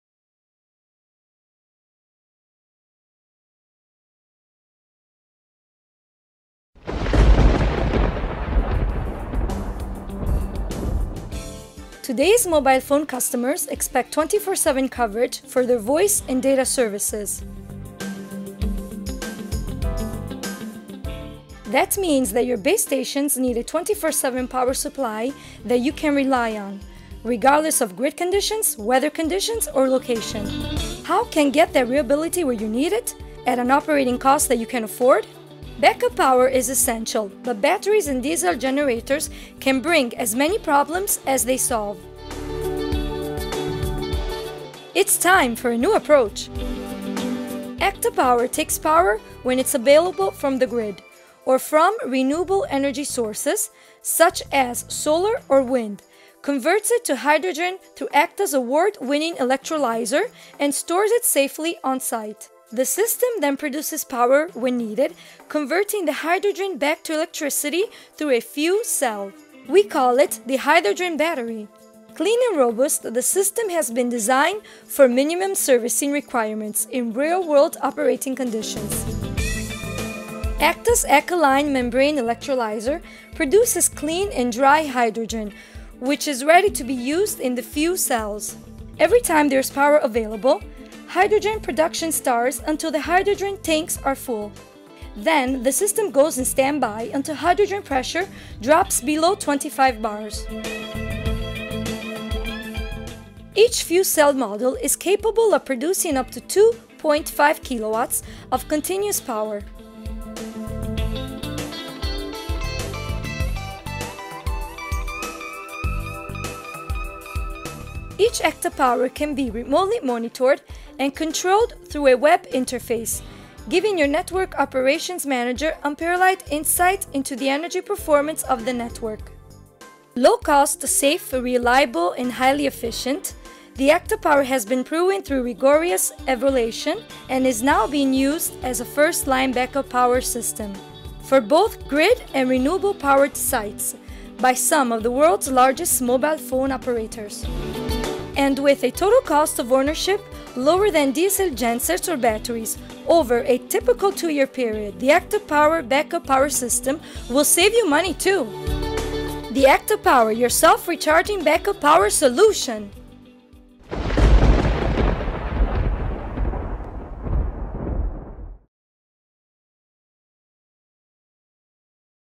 Acta Power back up power systems
Acta Power is an integrated hydrogen fuel cell back-up power solution.
Acta’s electrolysers allows hydrogen production to happen on-site, directly from renewables, using filtered rainwater, eliminating fuel logistics and making the system low maintenance and independent. There is no limit on how much hydrogen you can produce and store, so all the renewable energy can be fully optimized, as opposed to batteries, which are full after 3 hours.
With Acta’s integrated back-up power system, remote off-grid sites have a lower total cost of ownership than diesel generators and batteries.